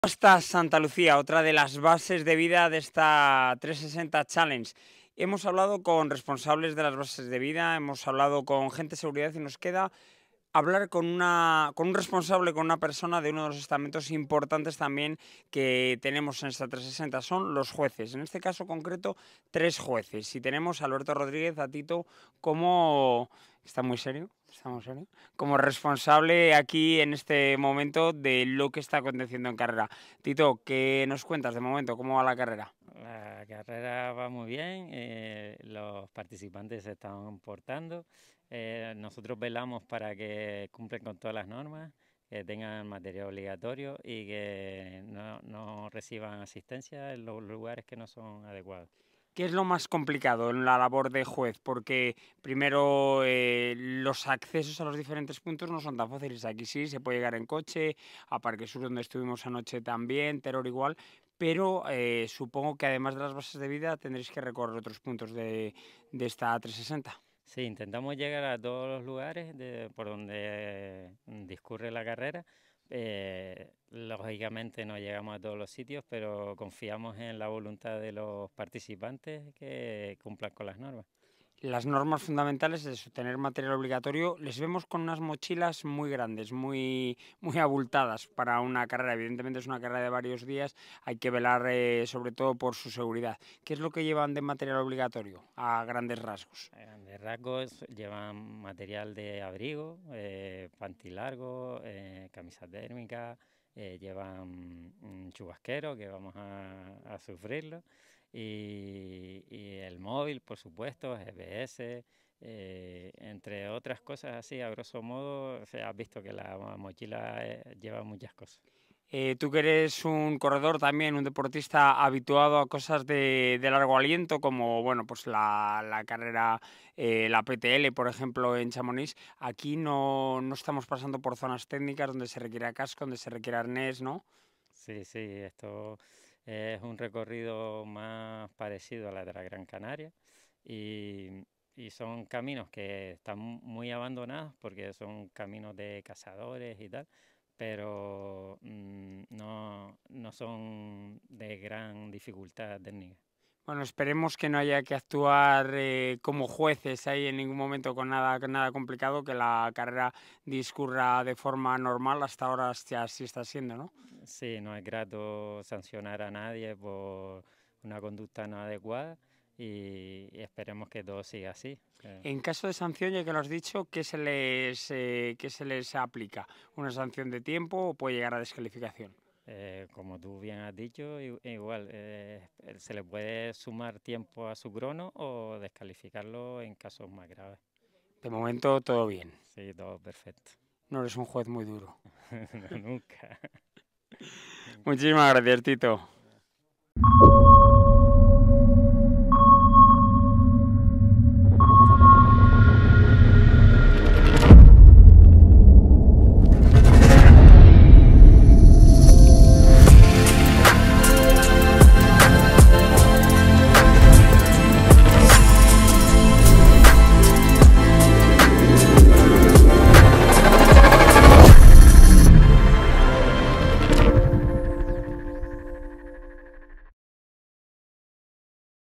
Entrevista Alberto Rodríguez, árbitro en la 360 The Challenge Gran Canaria
Entrevista Alberto Rodríguez, árbitro en la 360 The Challenge Gran Canaria, quien nos cuenta algunas de las cosas que los participantes llevan como material obligatorio. Los árbitros además de supervisar ese material también velan para que no haya incidentes ni trampas en el transcurso de la prueba